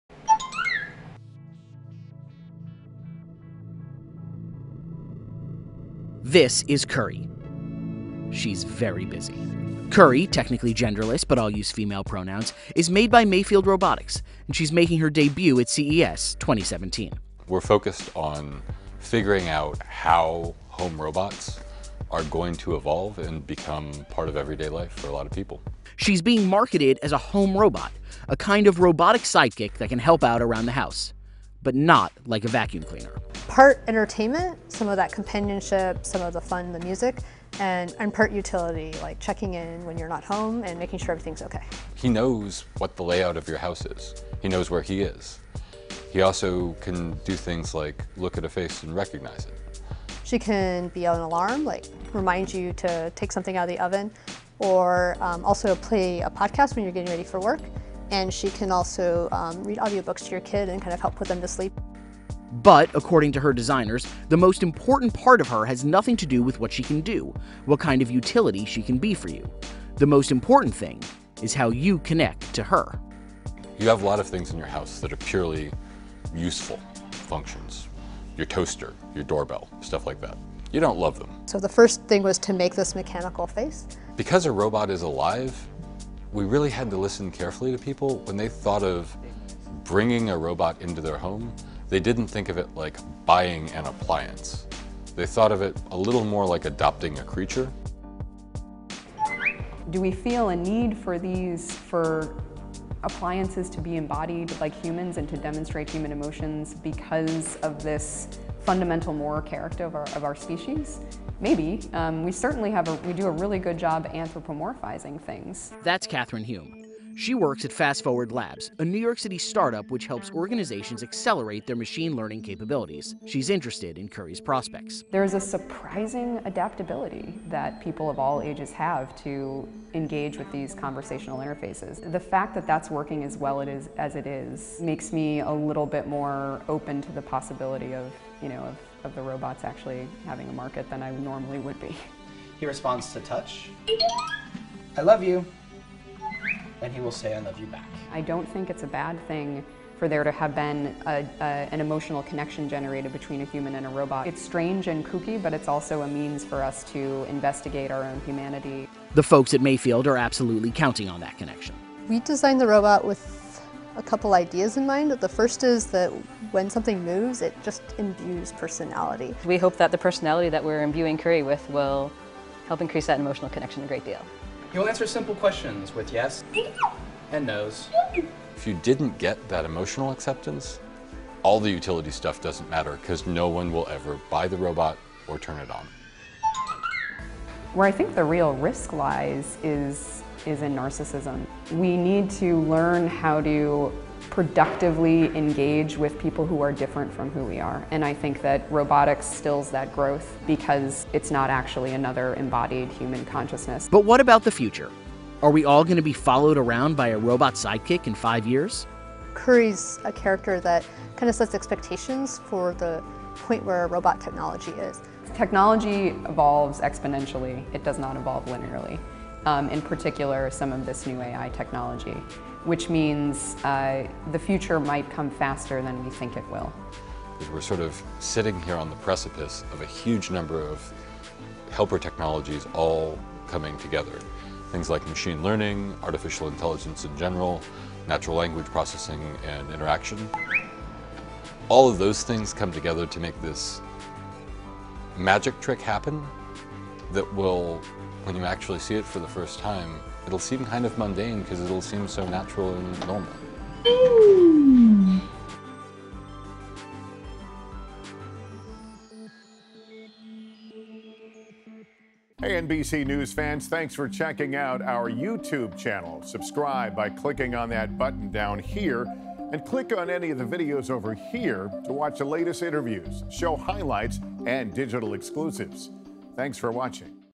Introducing Kuri, The Home Robot | Mach | NBC News
This is Kuri, a genderless home robot meant to bring a robotic spark of life to your home. Made by Mayfield Robotics, is she an attempt to make the future look like "the Jetsons" or is this the beginning of a world where robots do everything for us?